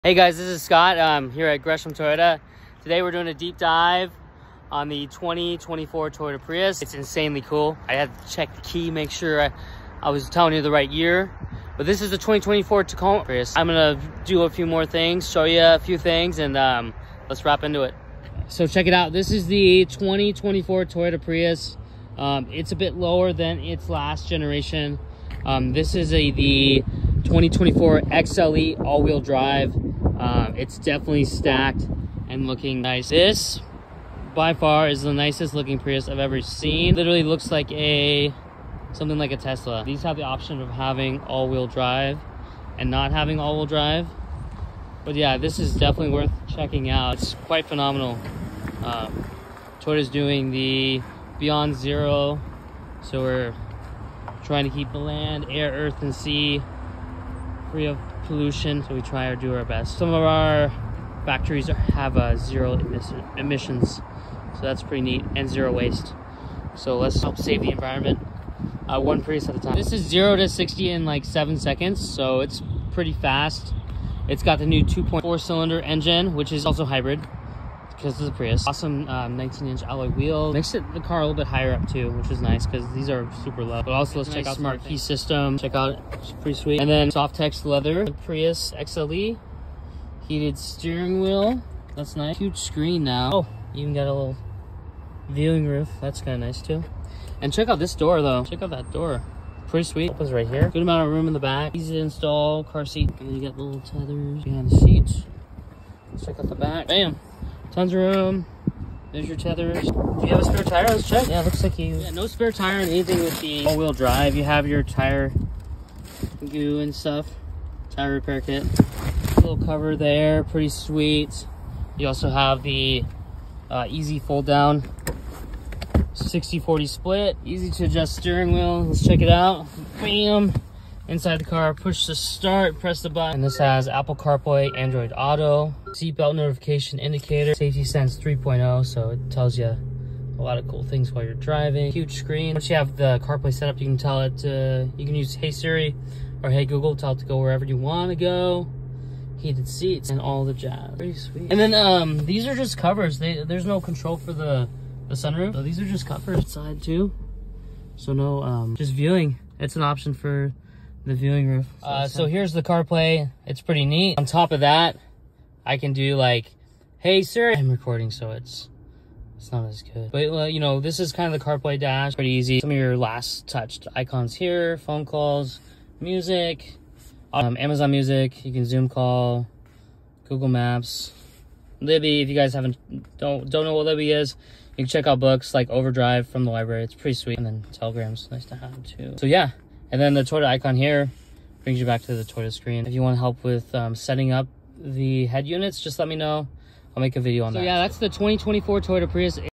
Get A First Look At The 2024 Toyota Prius With A Deep Dive At Gresham Toyota!
2024 Toyota Prius Deep Dive at

Check out the all-new 2024 Toyota Prius! We’re here at Gresham Toyota taking a deep dive into all the cool features that make this car so special. Don't miss out on the futuristic driving experience that the Prius has to offer.